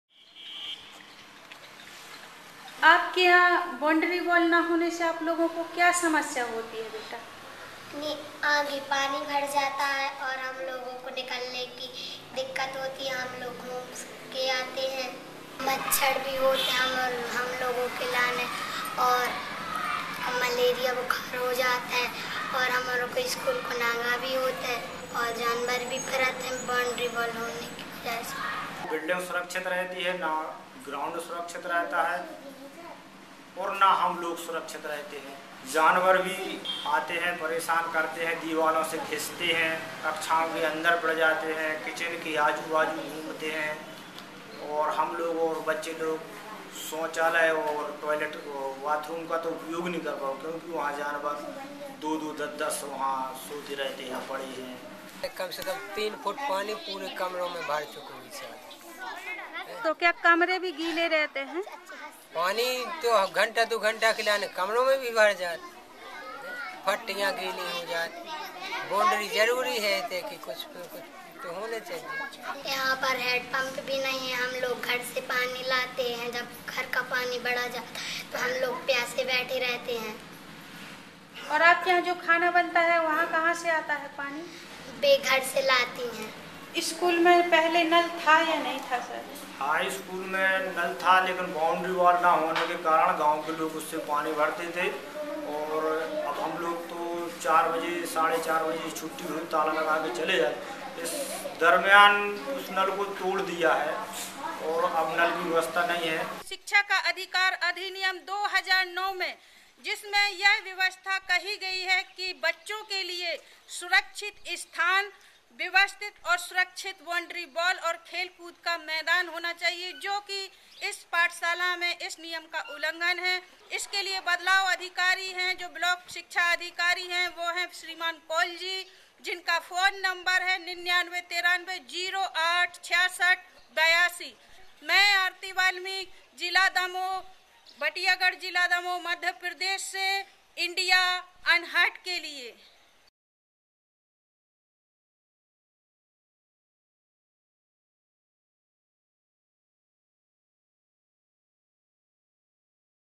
School With No Infrastructure | Arti reports for IndaiUnheard
Community correspondent Arti Bai Valmiki reports from Madhya Pradesh for Video Volunteers.